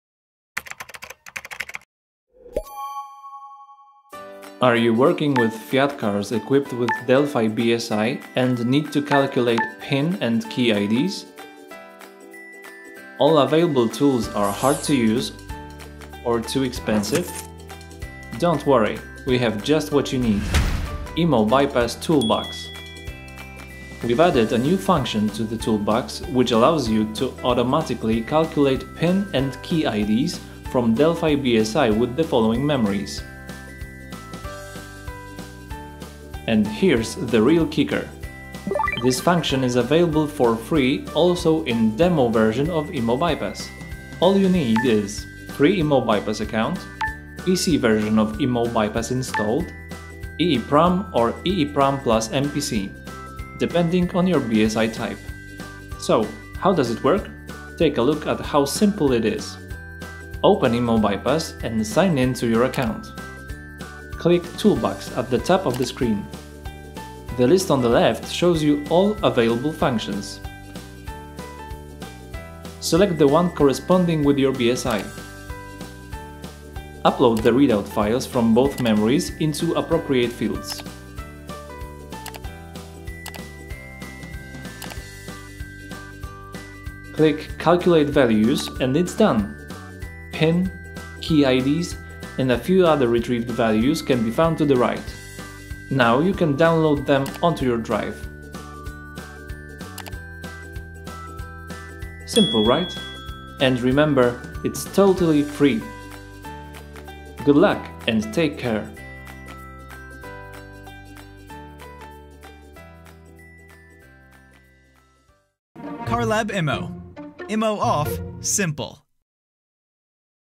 Delphi BSI (Fiat Group) PIN + KEY IDs | Free calculating in Immo Bypass Toolbox | CarLabImmo
Learn more / buy subscription:
Automatically calculate PIN and KEY IDs of your Delphi BSI from Fiat Group cars  using Immo Bypass Toolbox.

The tool is available for free also in DEMO version of Immo Bypass!